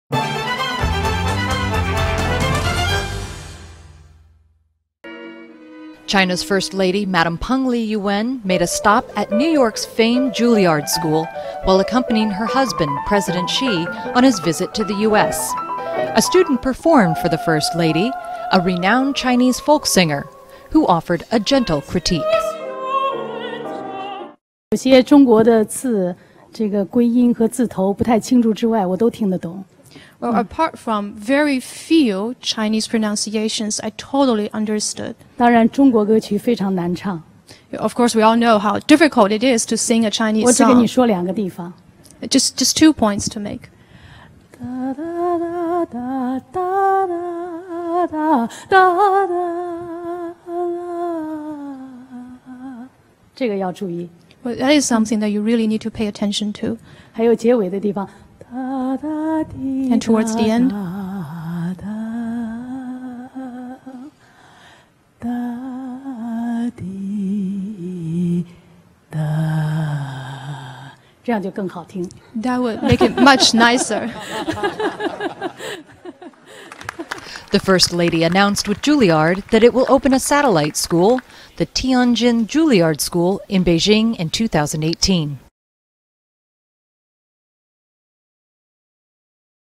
Peng Liyuan comments on performance and sings a few verses, too
During her visit to The Juilliard School in New York City on Sept 28, China's First Lady Peng Liyuan watched a Chinese folk song performance that prompted her to make a few comments.